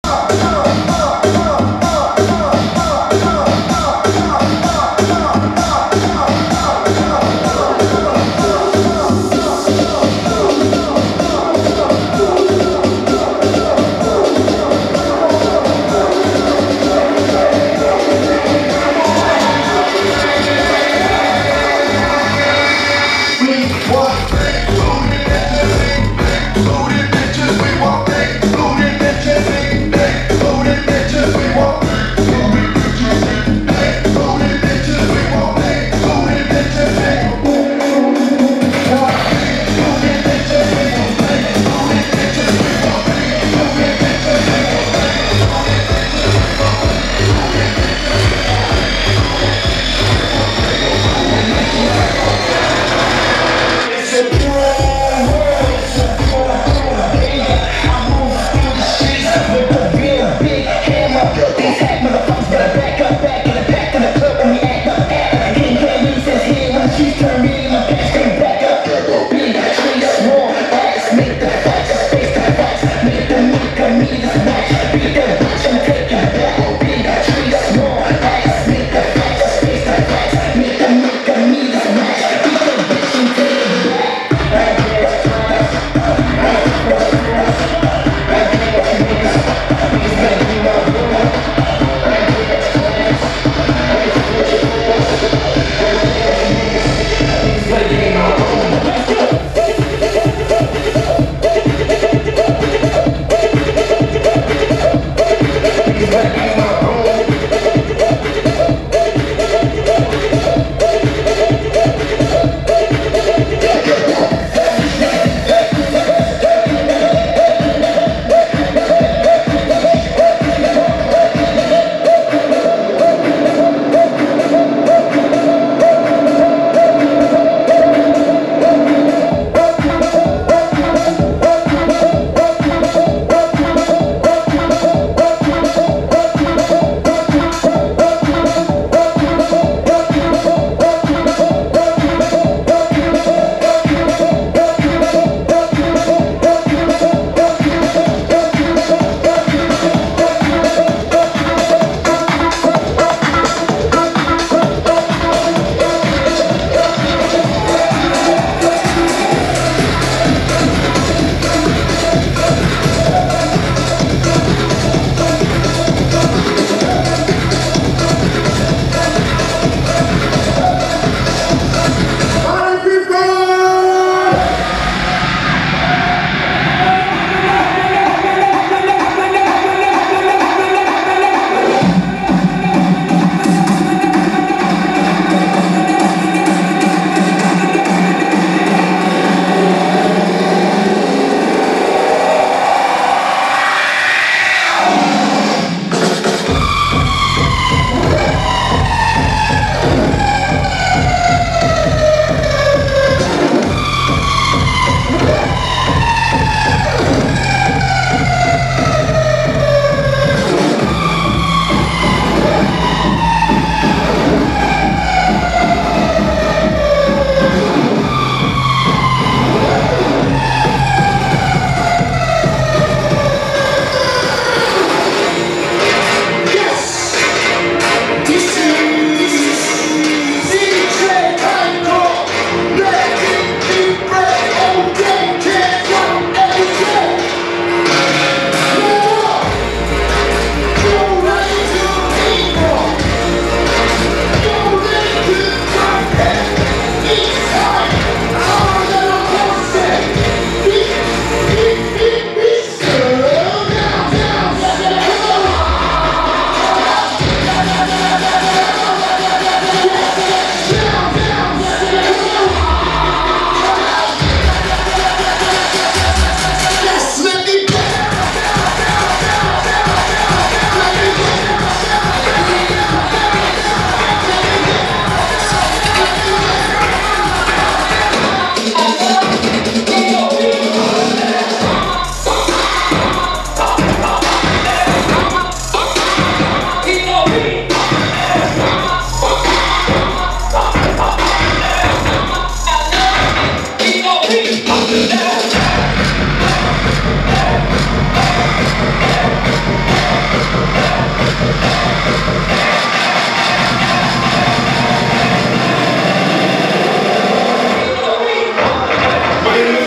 KINGMCK - JOIN THE DDND MVMT @ THE A, SEOUL - 12.6.2014
BNR in SEOUL THO!

So, I got a ticket last minute to Korea and made it in time to hang w/ Alex at The A in Seoul proceeded to get KOREAN TURNT

SO TITE

MY HOST/GUIDE IN KOREA AKA KINGMCK KILLLLIN IT ITAEWON STYLES